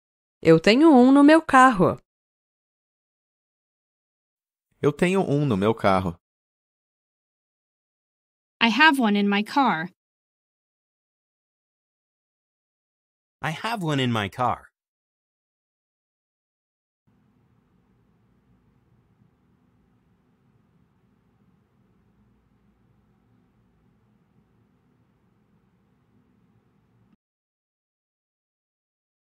How to say "I have one in my car." in Portuguese
Male Voice:
Eu tenho um no meu carro.

Male Voice Translation:
I have one in my car.